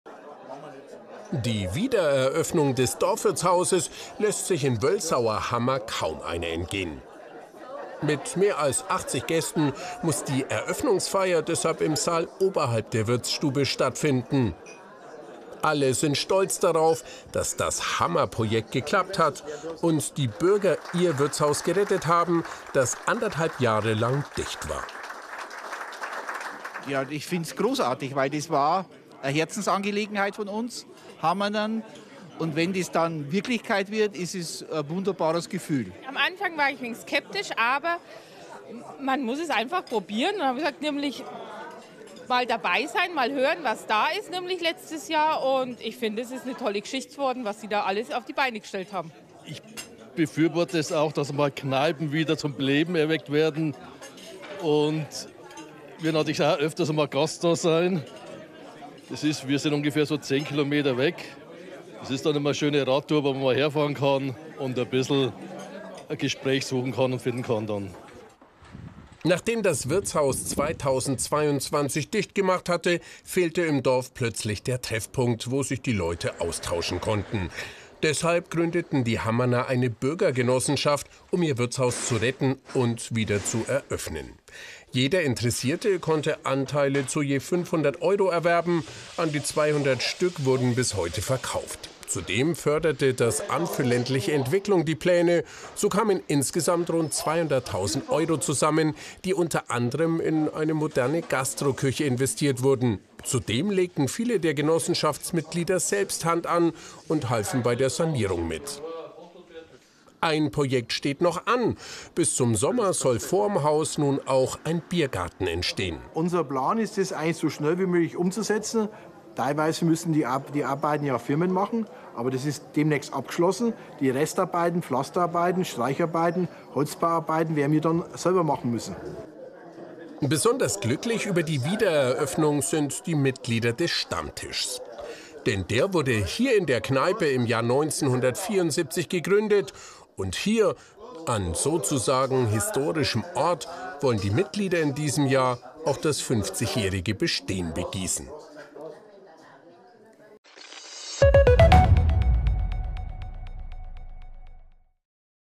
Dorfgemeinschaft packt an und rettet ihr Wirtshaus | BR24
Seit 2022 ist das Wirtshaus in Marktredwitzer Ortsteil Wölsauerhammer geschlossen. Doch der Stammtisch und die Dorfgemeinschaft haben die Sache selbst in die Hand genommen, zusammengelegt und die Kneipe gerettet.